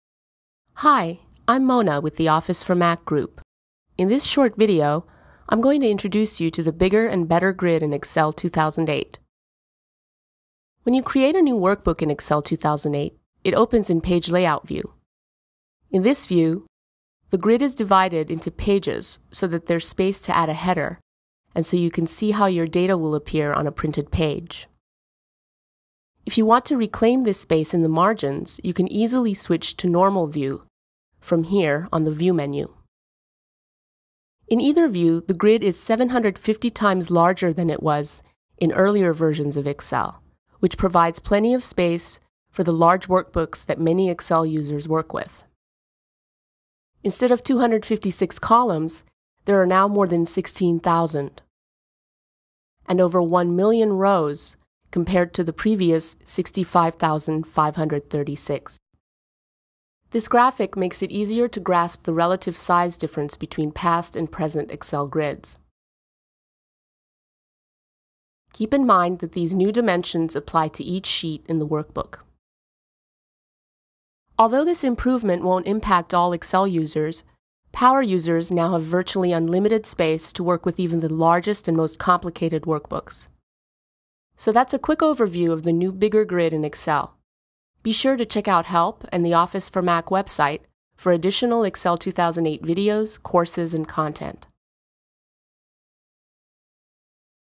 Excel 2008 New Grid Layout
Explore the bigger grid in Excel 2008.  Learn more about the print grid layout and normal layout functions.